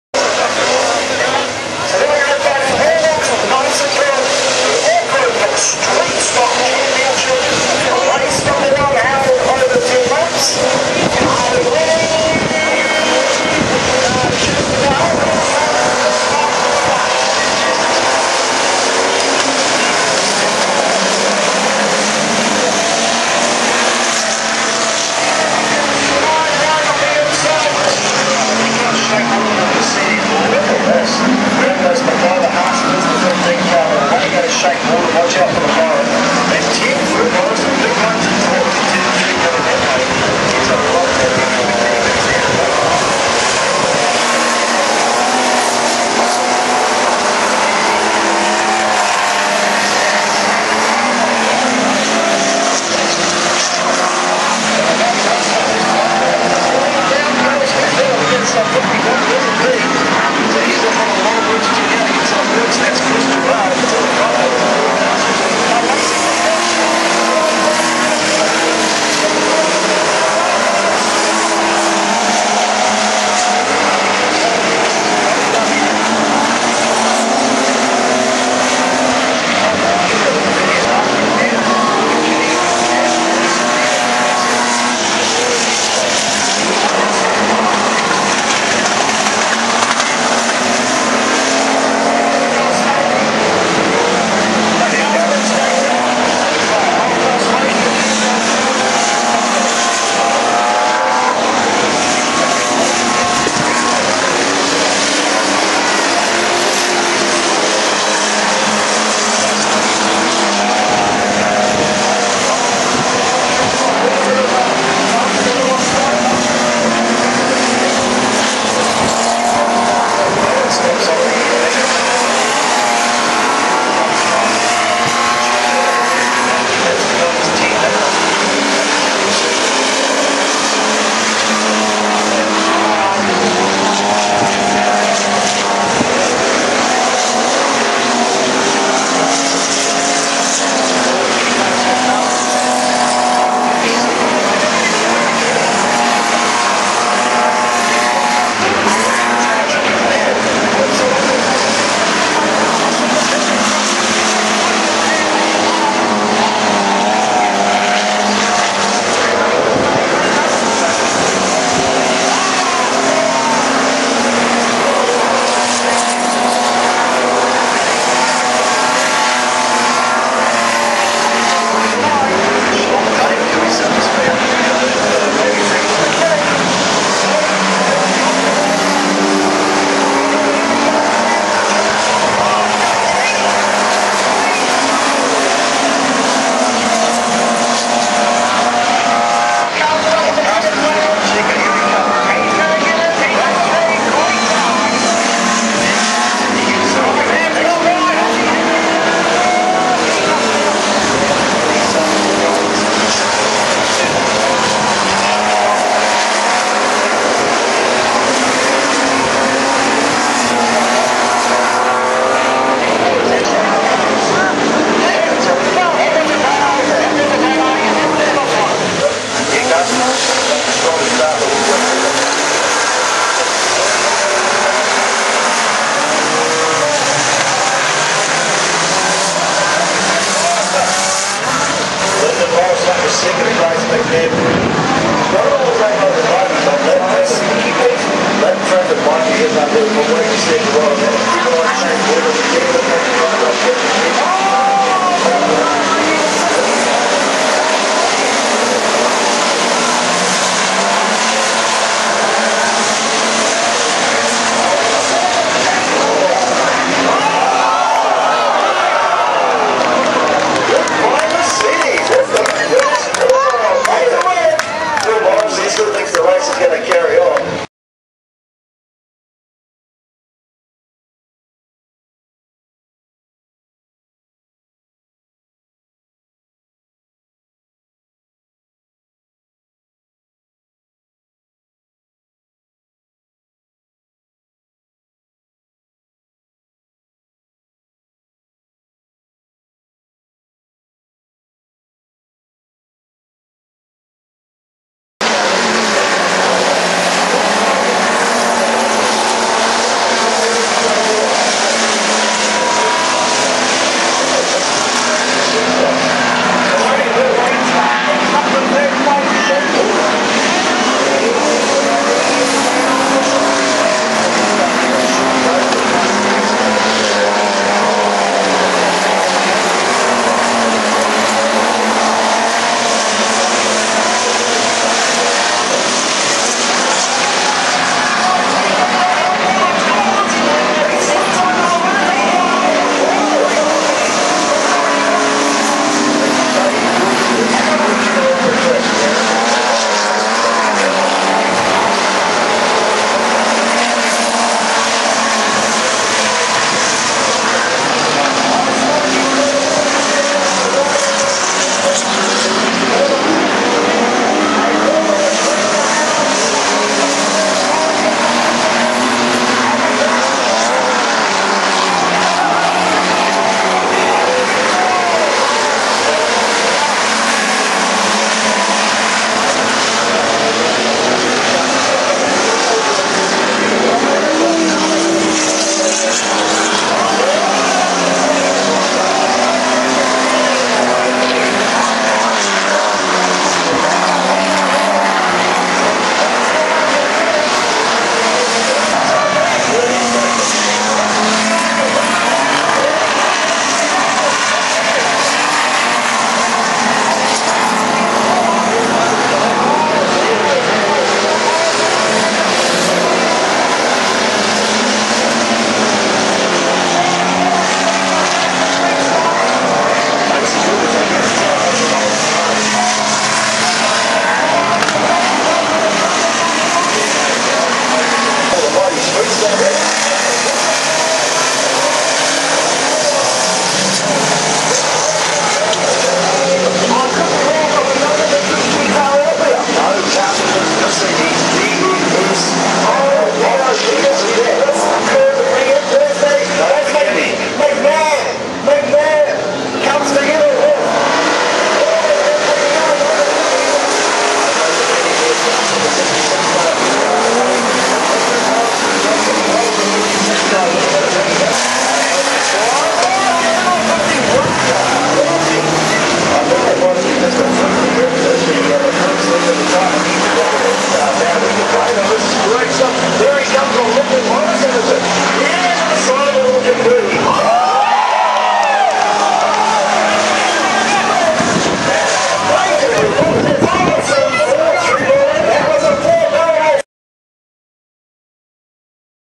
STREETSTOCK CHAMPS
The Annual Auckland Streetstock Champs created some close racing and one hell of a spectacular rollover